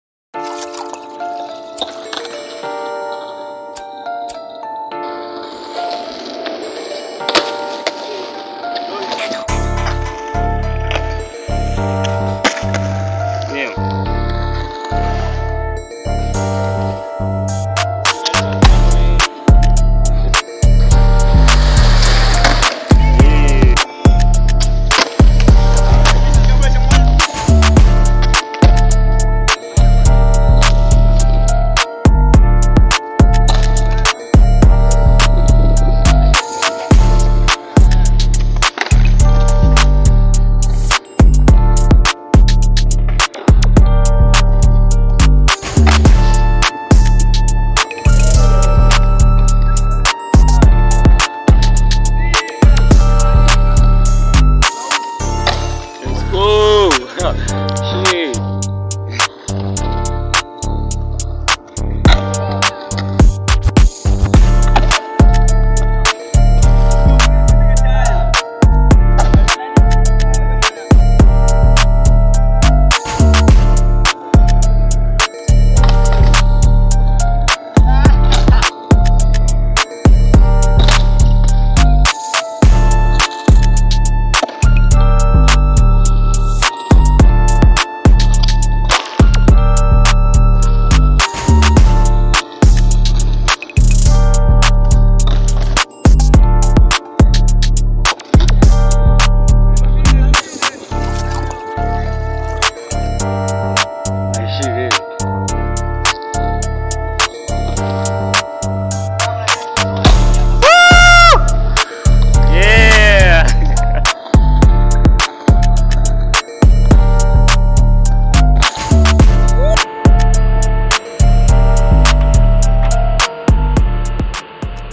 The shredders in my little town.
Meet Moses and Charles. These guys have huge passion for skateboarding and they are constantly progressing at it.
Elevated YEAR Eps 4.